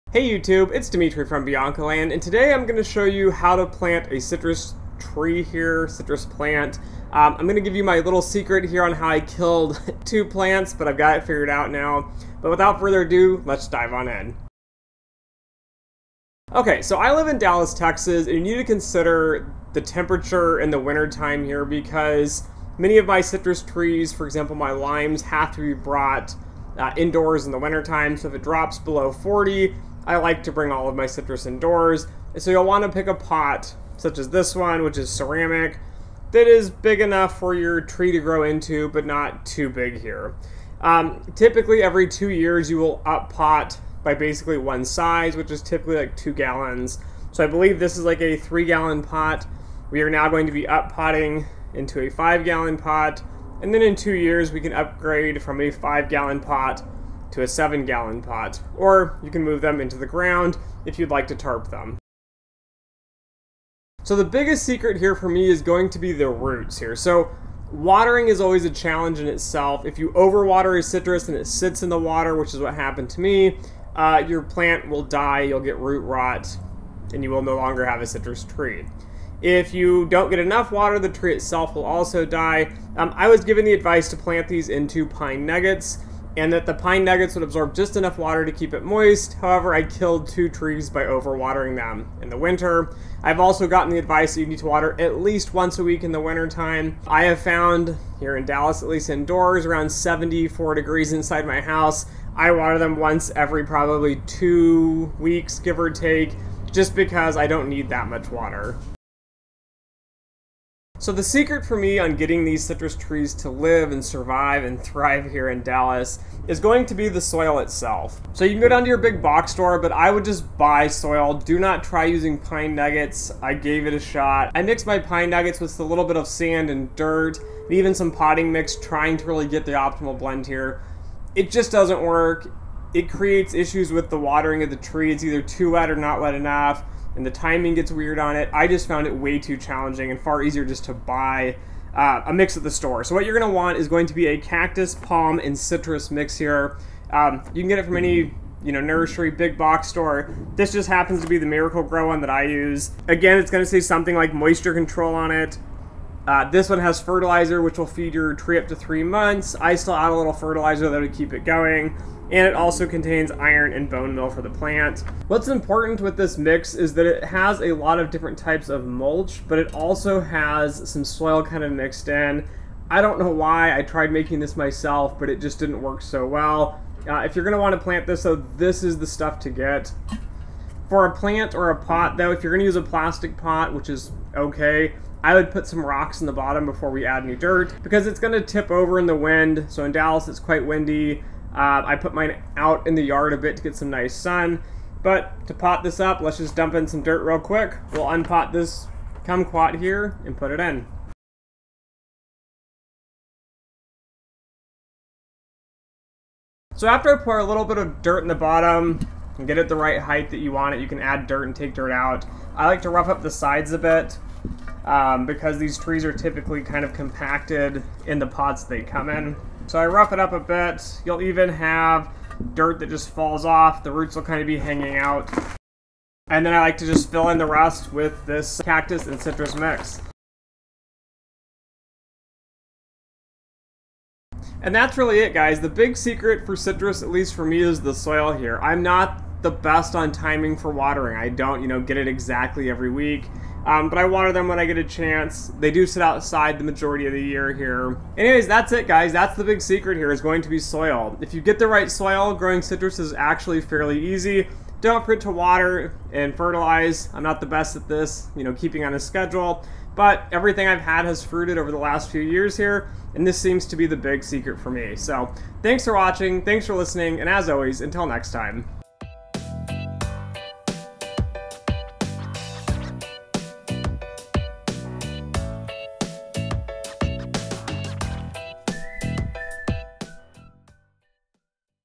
The Secret to Growing Citrus
What is my secret to growing citrus?

I learned it from killing a variegated pink lemon and a lime tree. I also had a Cara Cara orange that barely survived my care. Luckily after the first year and a lot of experimenting from online forums I decided to try a different dirt. It turns out citrus have very specific needs for soil. They can be tricky plants to care for as they have different needs in the summer versus the winter.

My citrus are grown in Dallas Texas however it does get too cold for most of them to be outside in the winter with no cover. I keep most of mine in pots and move them in doors for the winter. I just purchased an Orange Frost recently which should only require tarping in the winter for the first few years. Once it gets big enough it should be able to handle the cold here in Dallas.

Current Citrus I Own:
Improved Myer Lemon
Persian Lime
Cara Cara Orange
Orange Frost
Meiwa Kumquat